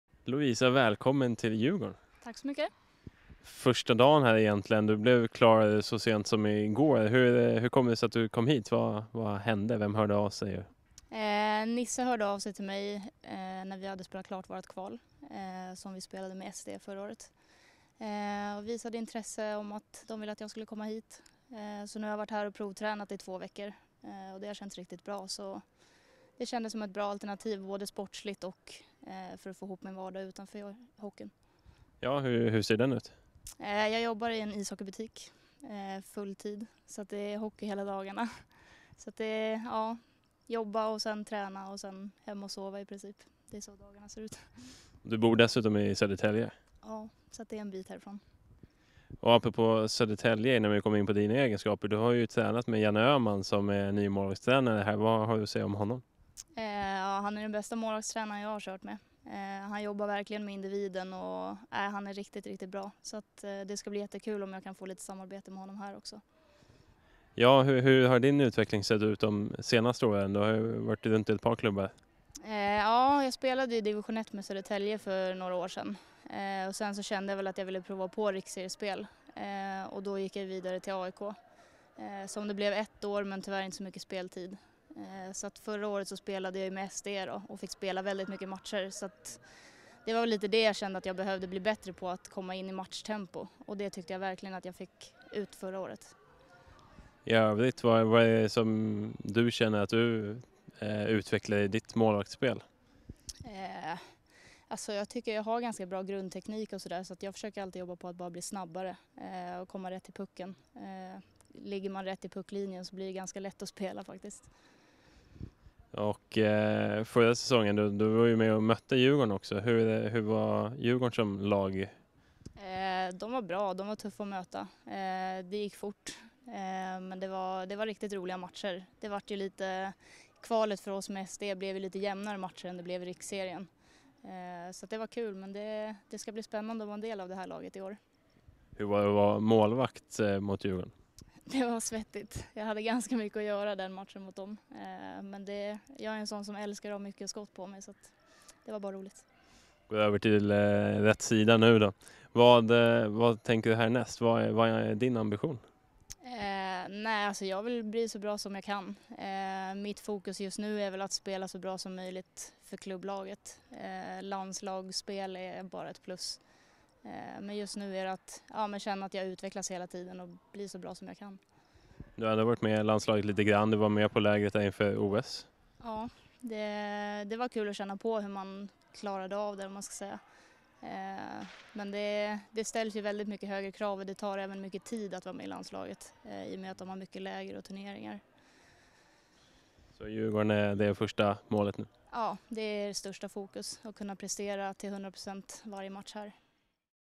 Ambitiös målvakt till Djurgården
Lovisa Berndtsson förstärker målvaktssidan.